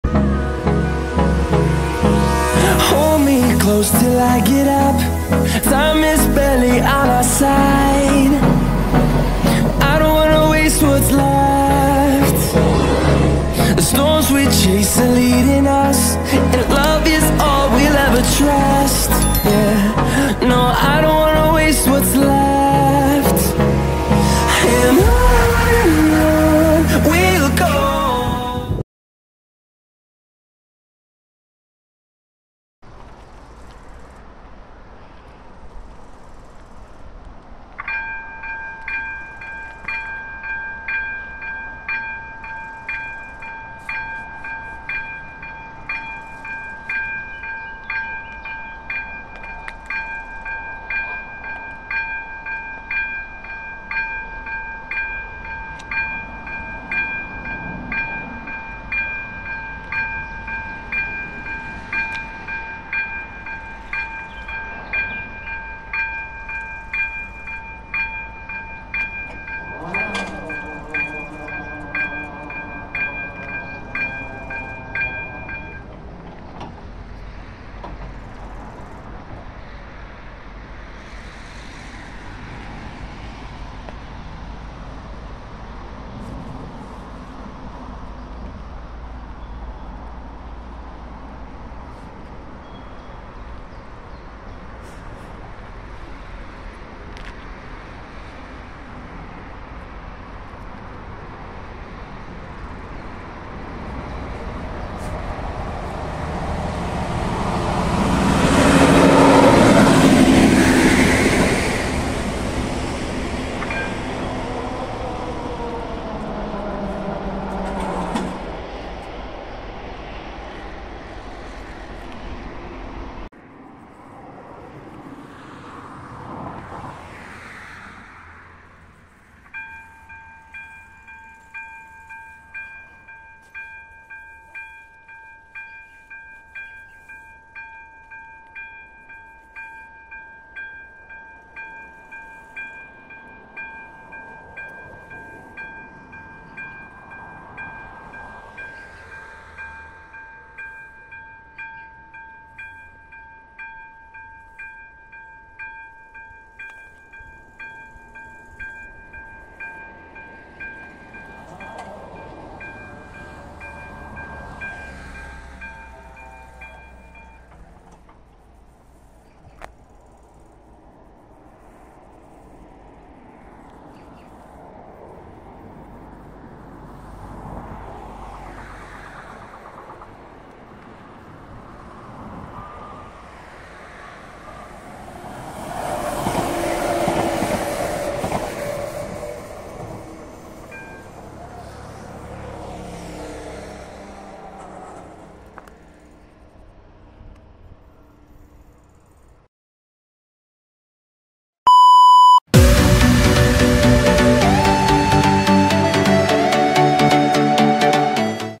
Railroad Crossing / Železniční přejezd Ústí n.L.-Střekov (CAROX) (P2966) 21/07/2024
Region / Kraj: Ústecký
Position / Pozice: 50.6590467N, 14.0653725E
Lights / Výstražníky: AŽD 97
Barriers / Závory: Yes / Ano
Bell / Zvonek: ZV-02
Edit: Me / Já
Thumbnail / Miniatura: Me / Já
Edited by / Editováno přes: CapCut
Thumbnail by / Miniatura udělána přes: Photopea
Date of Filming / Datum natáčení: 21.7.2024
Time of Filming / Čas natáčení: 14:10-14:40
by Adam nebo nevim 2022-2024